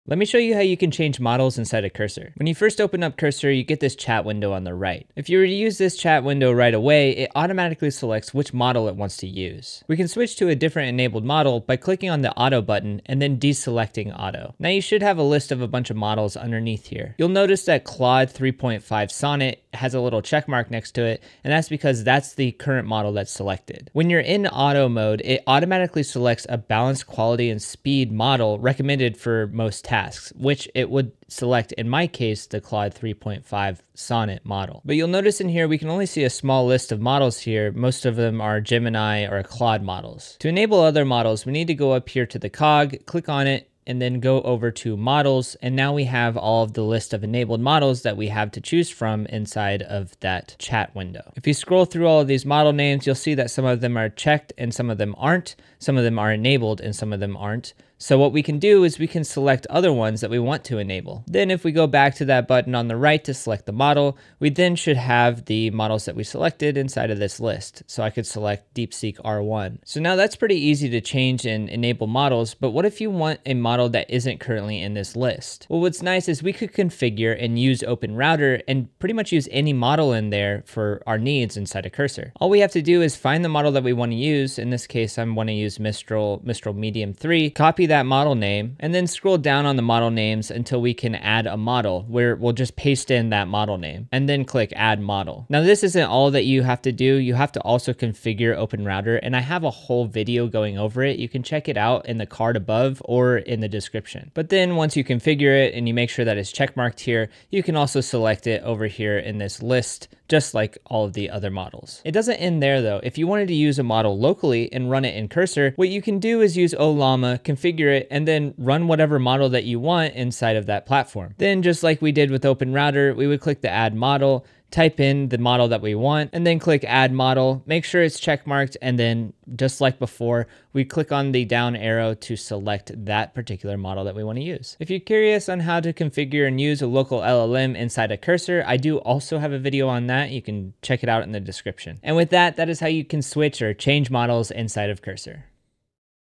How to Change Models in Cursor
I show you how to change models inside of Cursor AI